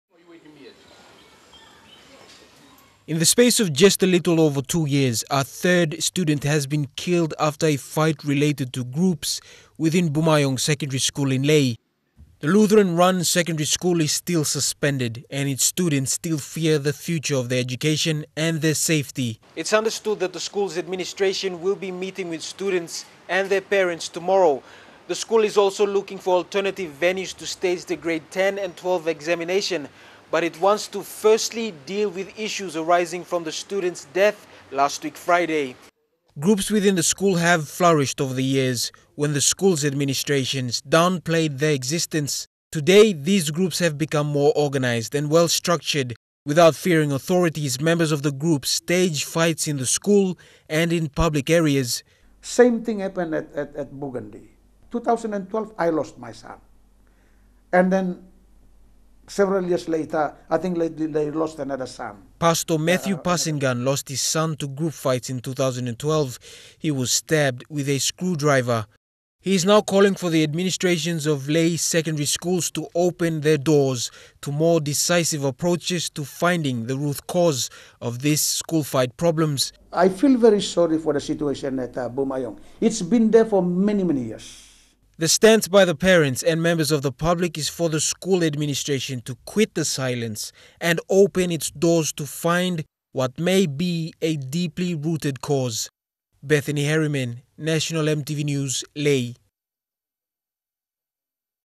Lae: Parents Of Slain Student Call For Action
The administrations’ of Secondary Schools in Lae need to expose cult groups with more decisive actions - visit us for the latest news...